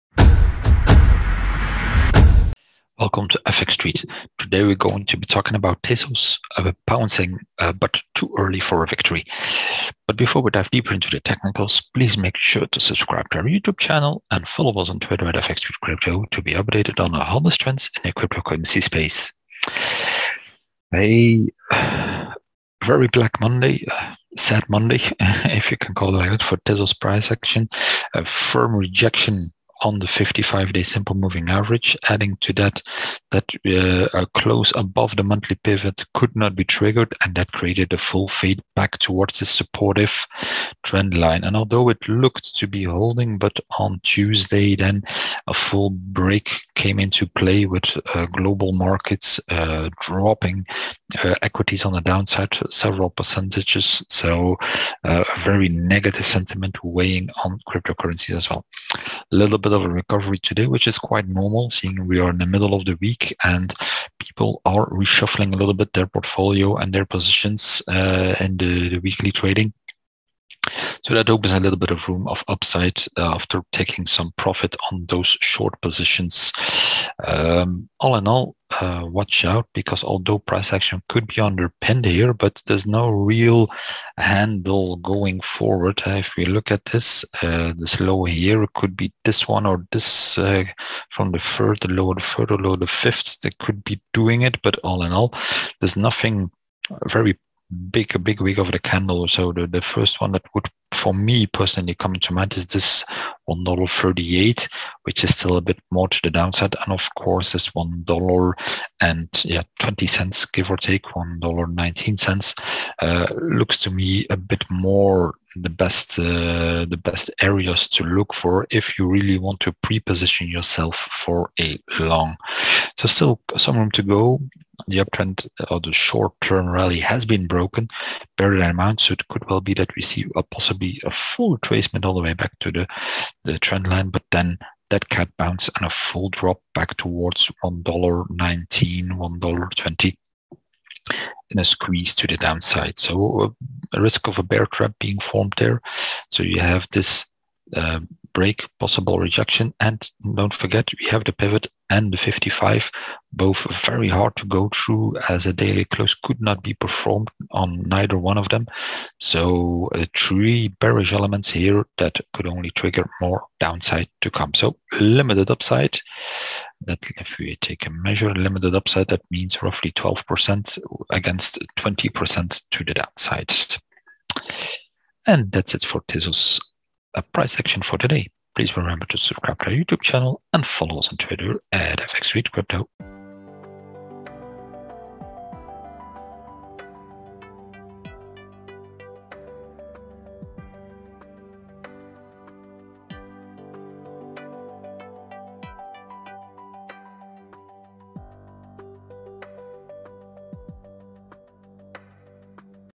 Tezos bouncing but too early for victory
A brief technical and on-chain analysis on Tezos price. Here, FXStreet’s analysts evaluate where XTZ could be heading next.